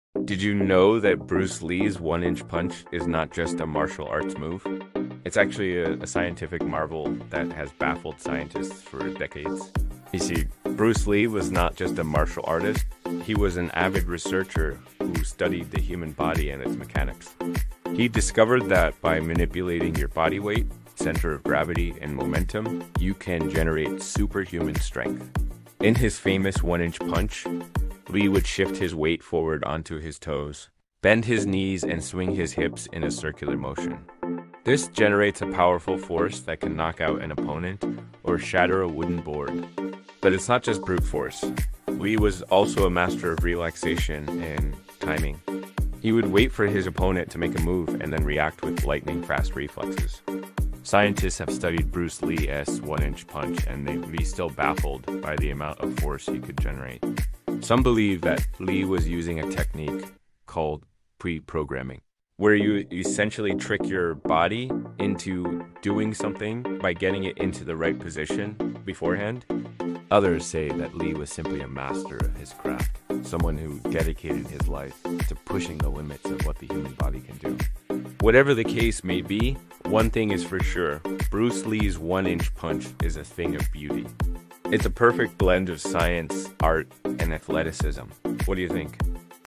Surprising Truth Behind Bruce Lee’s One Inch Punch- MYTH vs REALITY
Did you know that Bruce Lee's one-inch punch is not just a martial arts move?  It's actually a scientific marvel that has baffled scientists for decades.

You see, Bruce Lee was not just a martial artist, he was an avid researcher who studied the human body and its mechanics.

He discovered that by manipulating your body weight, center of gravity and momentum, you can generate superhuman strength.

In his famous one-inch punch, Lee would shift his weight forward onto his toes, bend his knees and swing his hips in a circular motion.

This generates a powerful force that can knock out an opponent or shatter a wooden board.

But it's not just brute force. Lee was also a master of relaxation and timing.
He would wait for his opponent to make a move and then react with lightning-fast reflexes.

Scientists have studied Bruce Lee 's one-inch punch and they 're still baffled by the amount of force he could generate.

Some believe that Lee was using a technique called pre-programming, where you essentially trick your body into doing something by getting it into the right position beforehand.

Others say that Lee was simply a master of his craft, someone who dedicated his life to pushing the limits of what the human body can do.
Whatever the case may be, one thing is for sure, Bruce Lee's one-inch punch is a thing of beauty.

It's a perfect ble#n of science, art and athleticism.